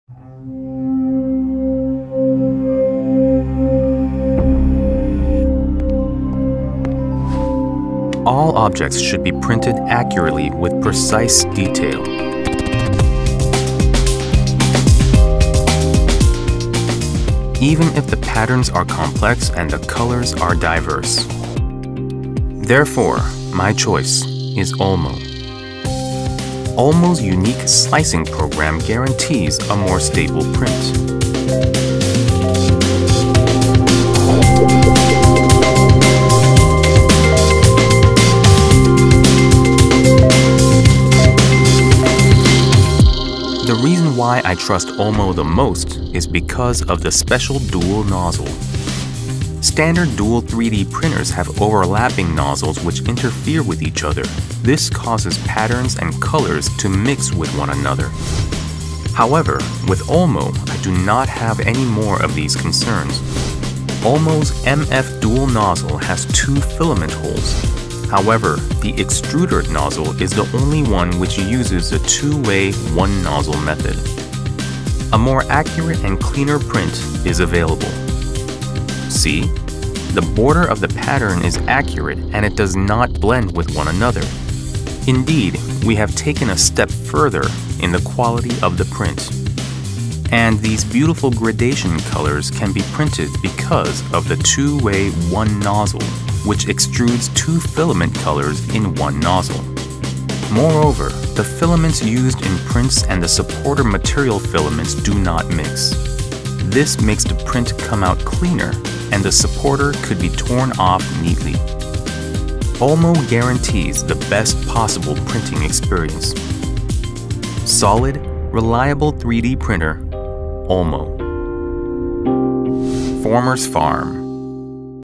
3D 프린터 올모
포머스팜의 3D 프린터 OLMO는 2 way 1 nozzle 이라는 특수한 노즐을 가진 듀얼 출력 프린터 입니다. 깨끗한 듀얼 출력은 물론 아름다운 그라데이션 모드까지 가능한 동급 대비 최강 듀얼 출력 퀄리티를 자랑합니다.